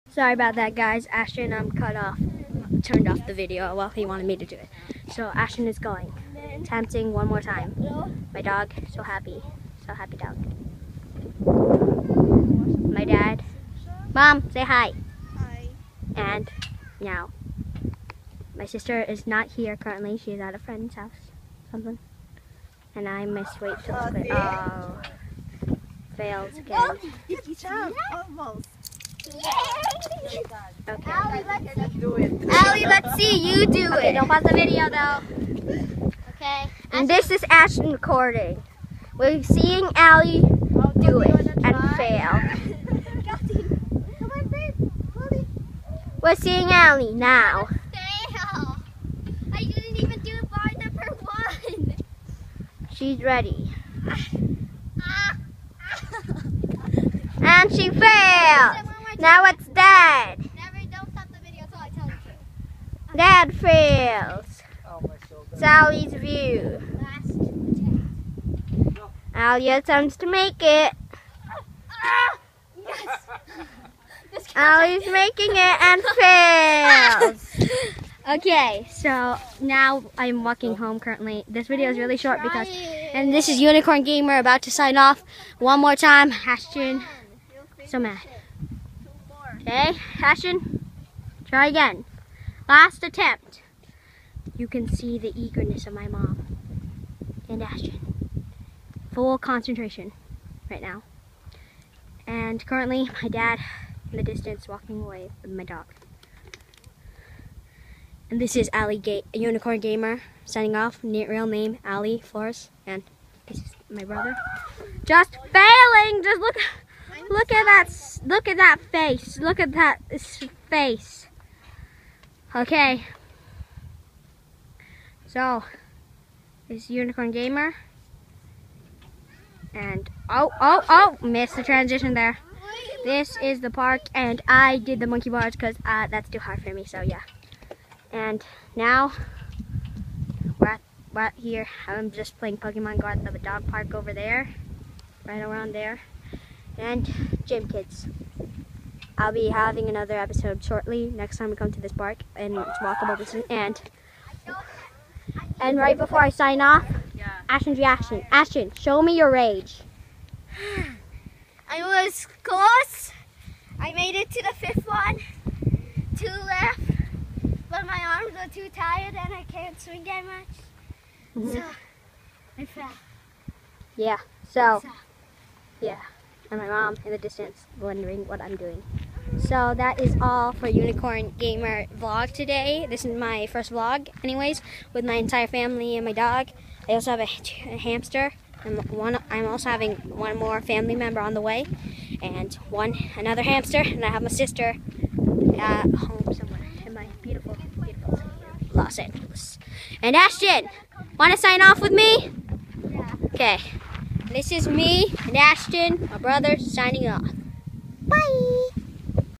Unicorn gamer gym kids part 2
Gym kids a blog of my family and I to the park and do fun gym stuff